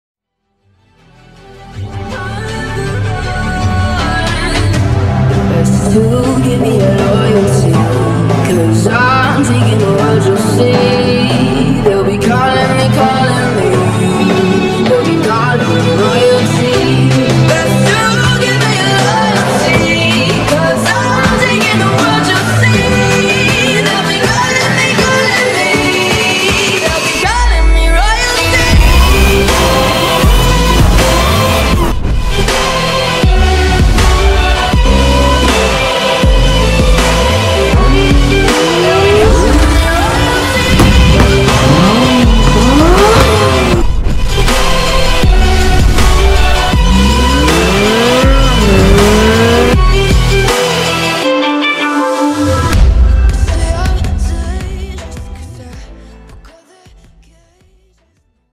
A message for car enthusiasts
"electric vehicles are the future"
send this video to every car guy you know. any car enthusiast that supports the combustion engine. ICE's will live on, its not over yet.